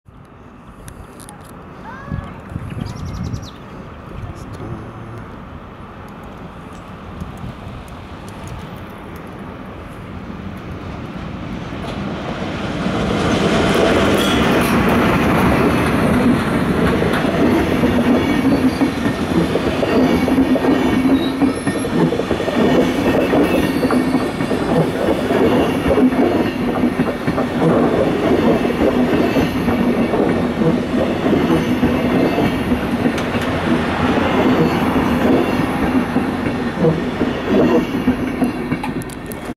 ARRIVEE DE L EC 195 A LINDAU
A l'entrée de la gare de Lindau, arrivée de l'EC 195 Zurich-Munich tracté par une RE 421 jusque Lindau. (avril 2018)